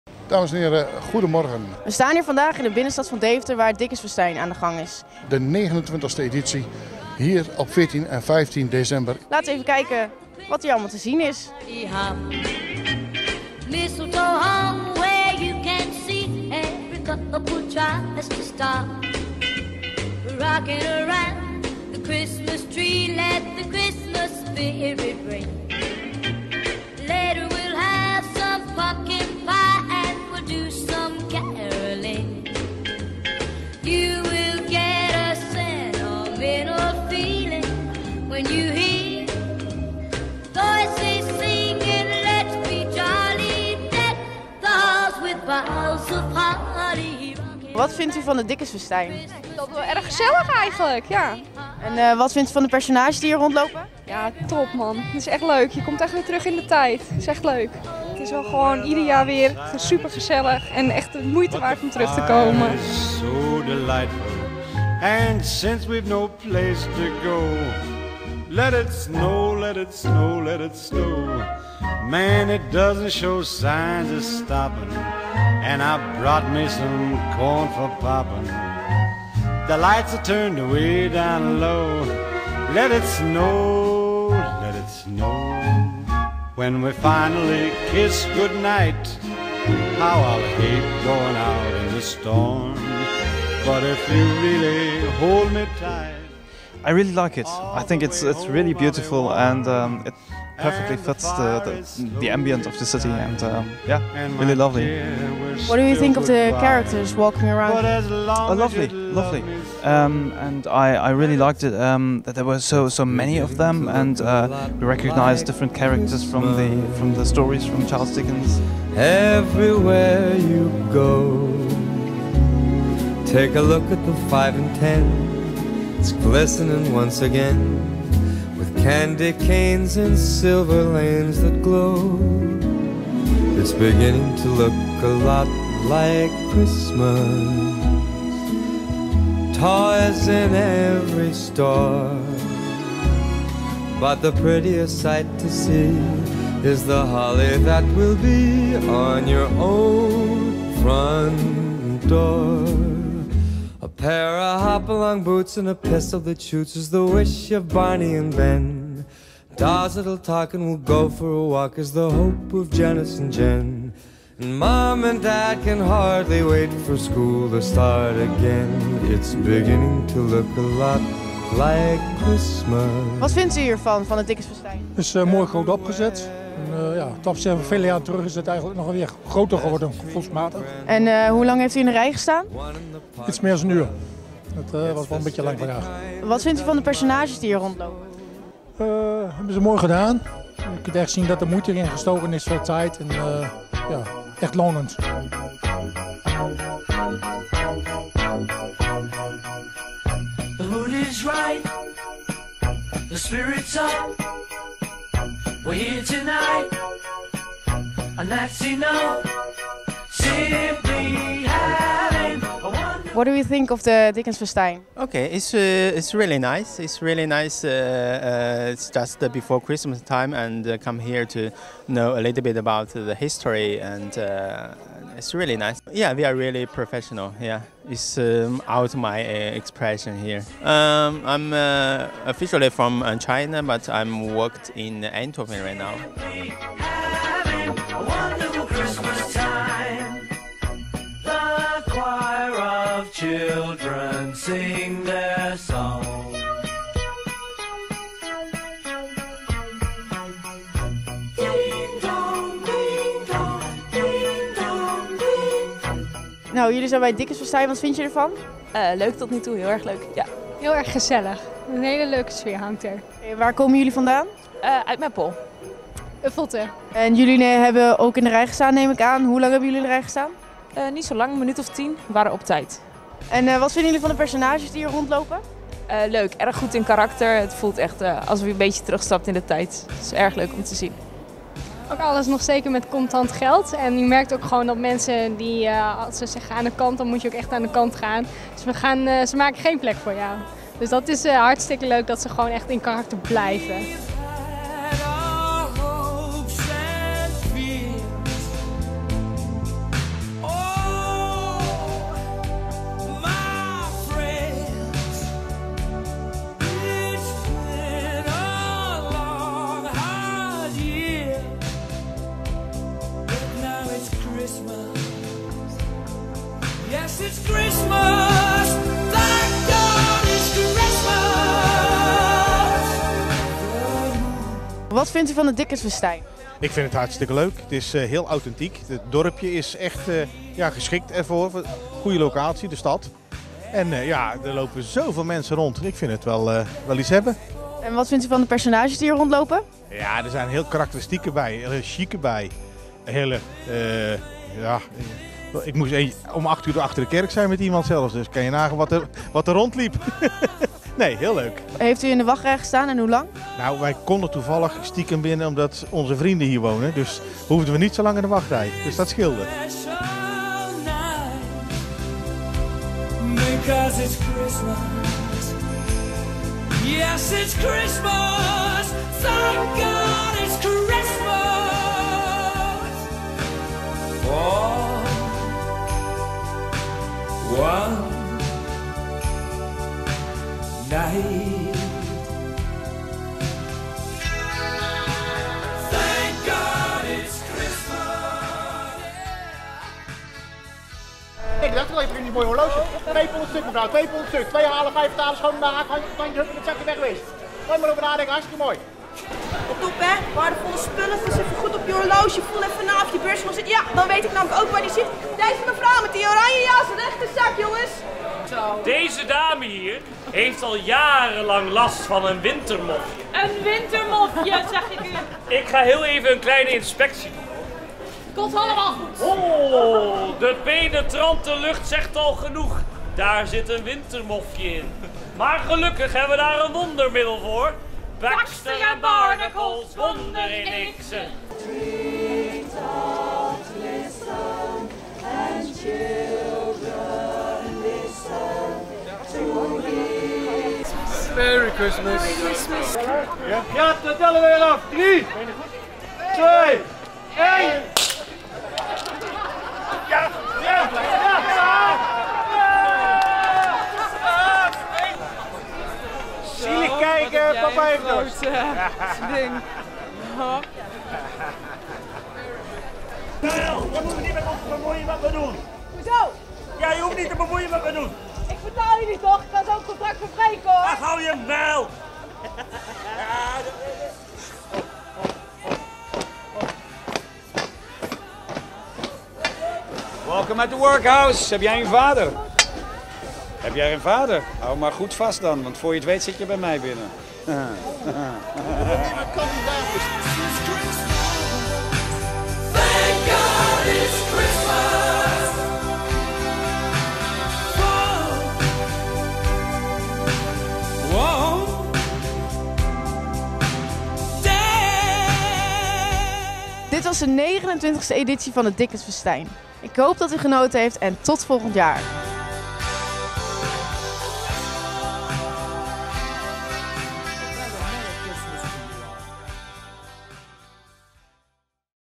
Dickens Festijn sfeerimpressie
Het was weer zover, dit weekend was de 29e editie van het Dickens Festijn. Het festijn komt jaarlijks weer terug met de sfeer en de stijl van 1800. Er lopen minstens 950 verschillende personages uit de boeken van Dickens rond, wij van Deventer Radio Televisie waren er ook bij!